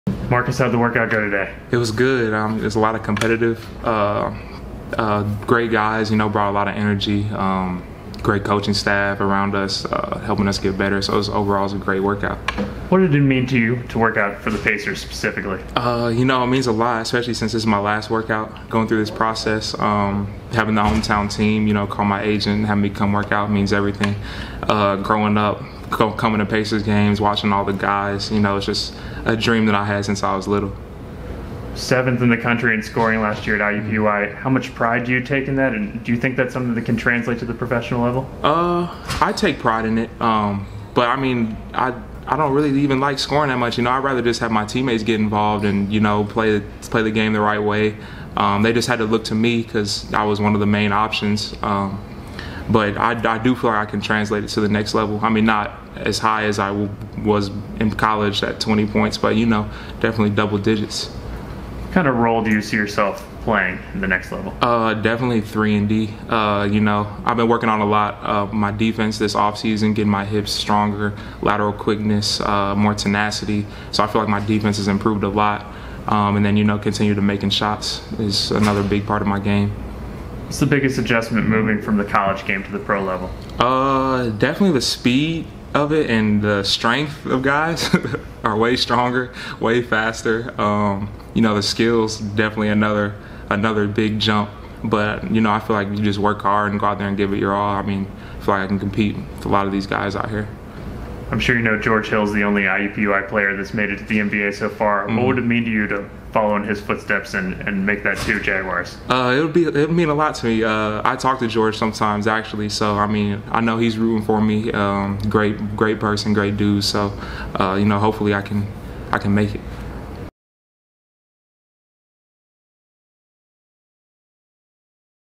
2021 Draft Workouts: Marcus Burk
July 26, 2021 - IUPUI guard Marcus Burk speaks with Pacers.com's Wheat Hotchkiss following his pre-draft workout with the Pacers at the Ascension St. Vincent Center.